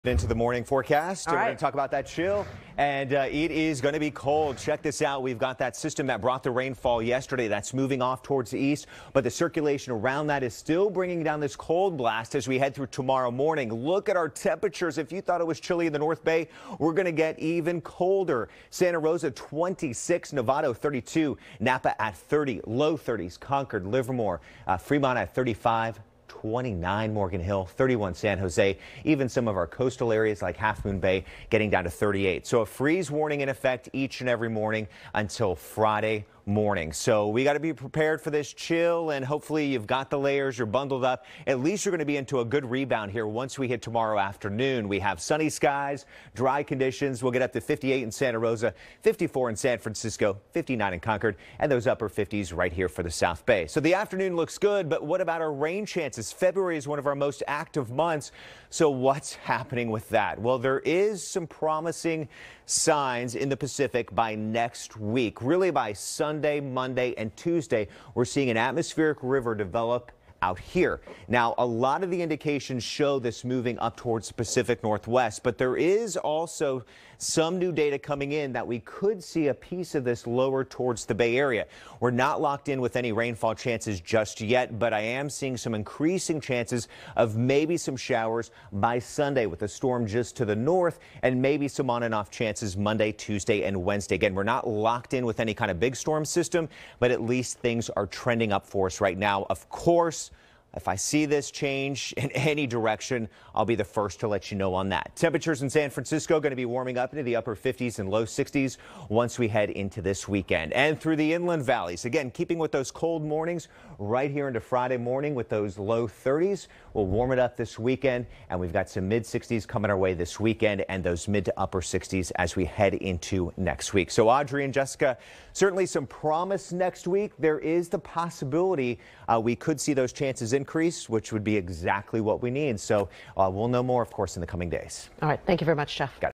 Bay Area Forecast: Freezing Morning Ahead
Chief Meteorologist Jeff Ranieri has the latest on the freezing temperatures ahead in your Microclimate Forecast.